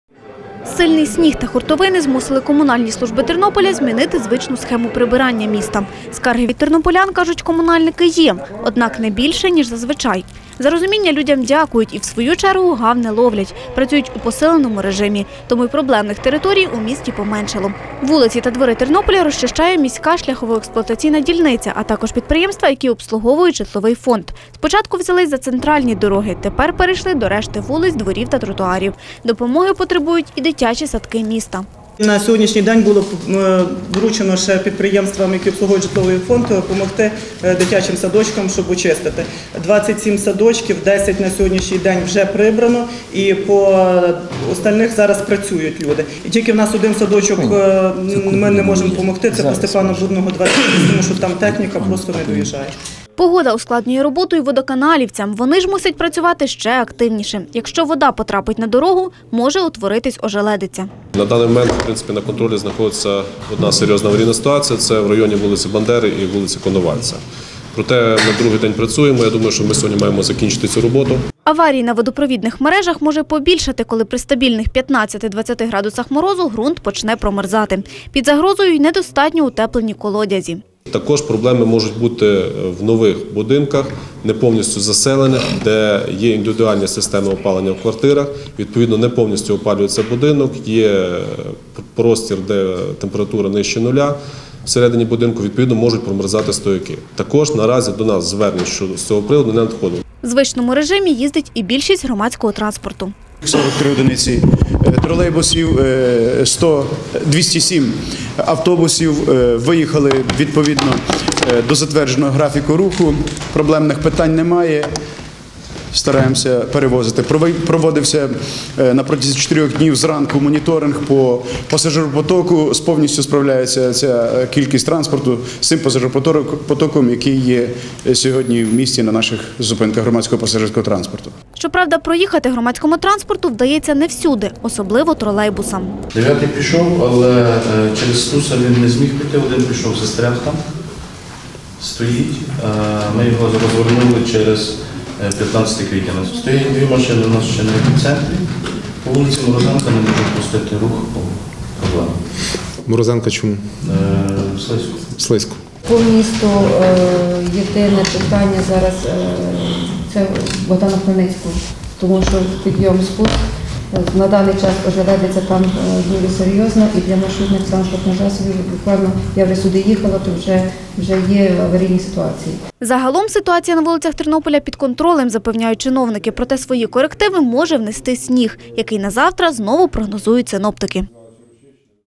Комунальники просять не залишати авто на вулиці
Не залишати автомобілів на вулицях та у дворах будинків просять комунальники тернополян. Зайвий транспорт заважає їм розчищати засніжені дороги. Попри це, після вчорашнього снігопаду більшість вулиць та дворів міста уже прибрані. Адже зі сніговими заметами комунальні служби Тернополя борються спільно.